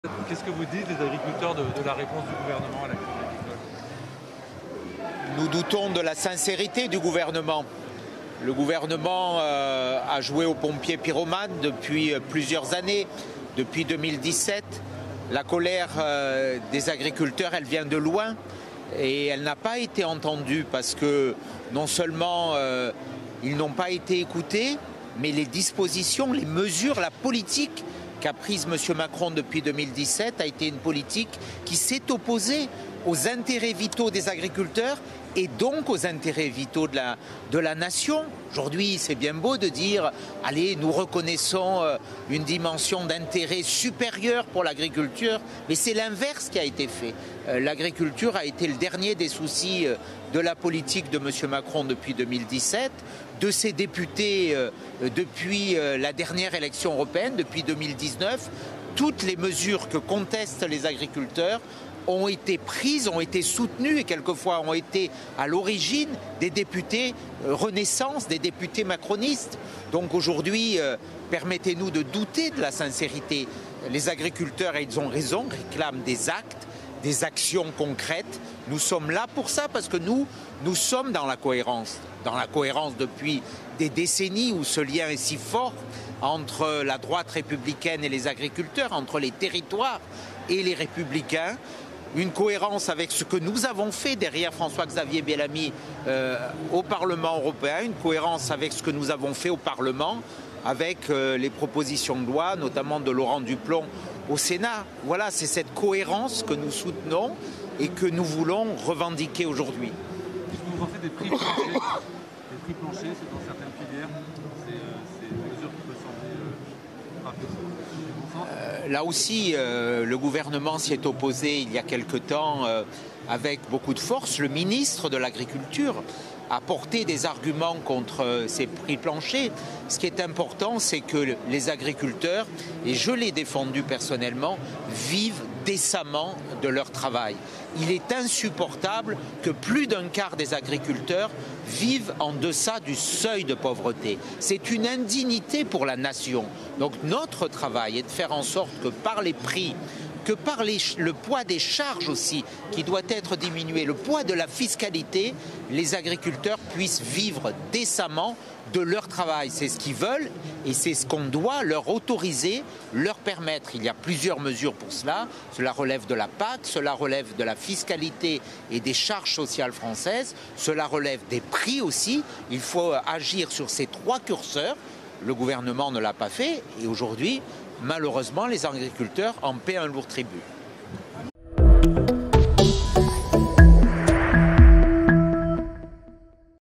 Au Salon de l’agriculture, Éric Ciotti s’interroge sur "la sincérité du gouvernement"
Le président des Républicains s’est rendu au Salon de l’agriculture ce lundi. Au micro de Public Sénat, il s’interroge sur les annonces faites par l’exécutif pour calmer la grogne du monde rural, estimant que les mesures qui sont aujourd’hui dénoncées par les agriculteurs découlent directement de la politique conduite par Emmanuel Macron depuis 2017.